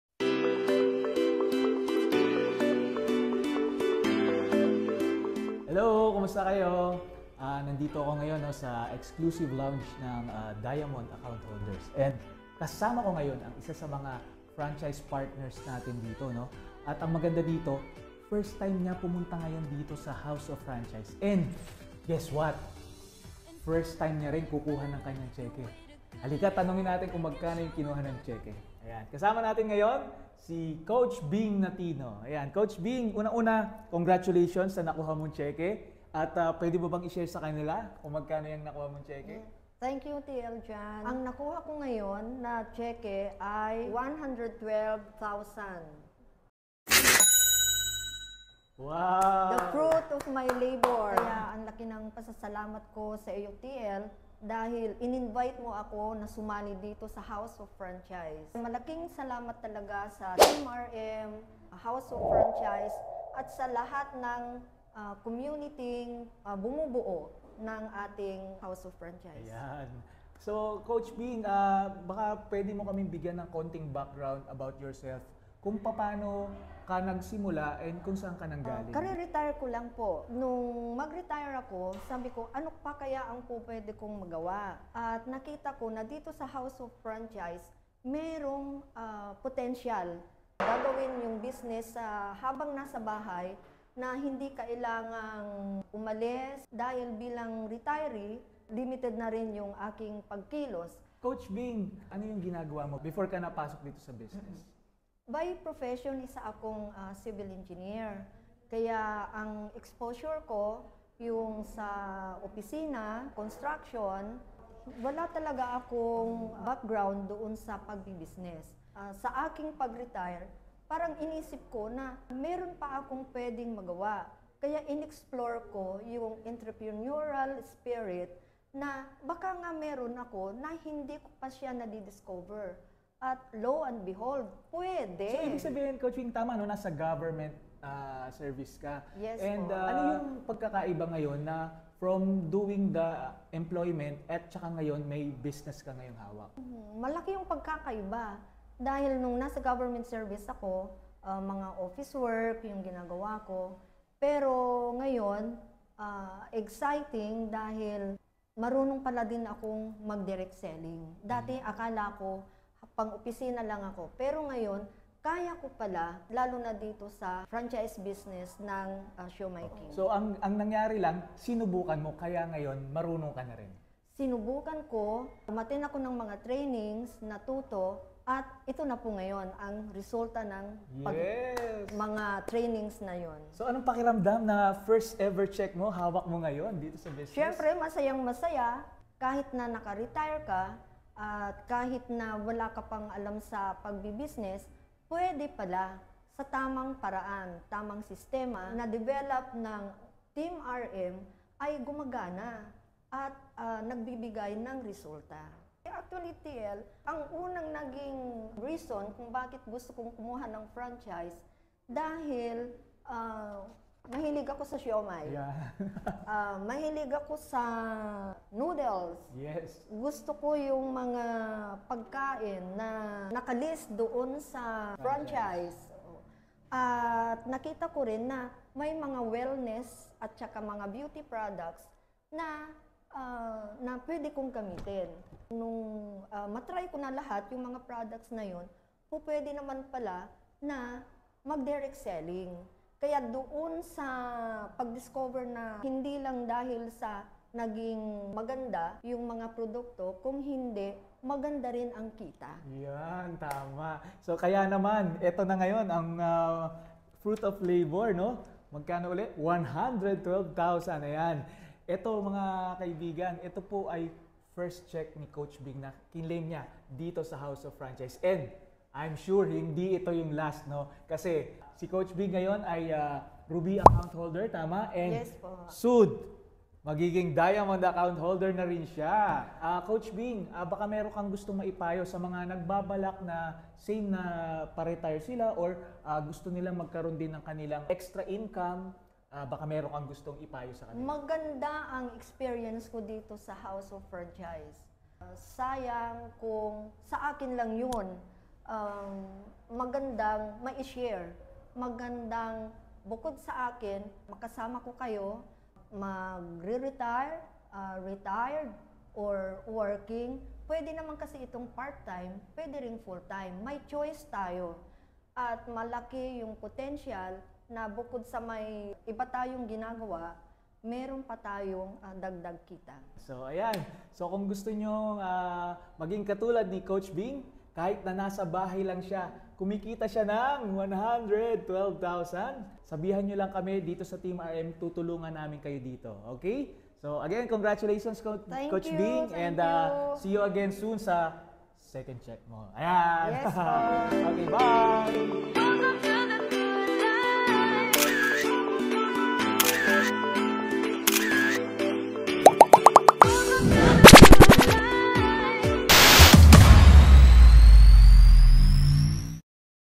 Retired Civil Engineer, Kumita ng 6-digit Income Kahit Nasa Bahay Lang!
Isang retired Civil Engineer from the Government Service, kumita ng 6-digit income kahit nasa bahay lang! 😱🤩🤑

Watch the never-before released, EXCLUSIVE Interview of Coach Araceli Bing Natino as she share her thoughts on how she got her 1st ever check at The House Of Franchise even though she had no experience in handling Food and Franchising Business. 🎤📺 👀

🎥 by Malou Layos-Manalastas